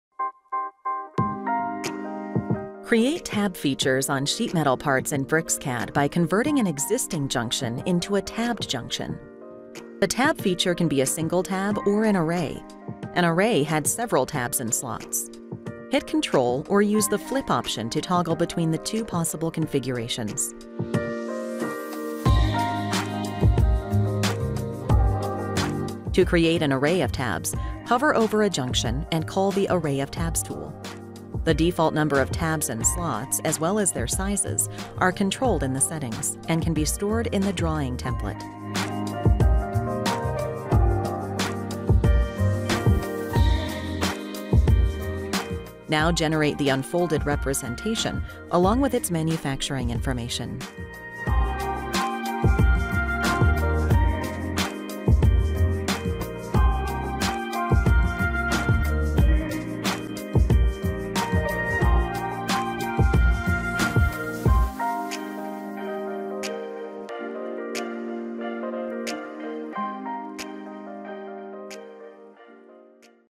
How to Create Tabs - BricsCAD Mechanical
In this video, you will learn how to add slots and tabs to your sheet metal parts.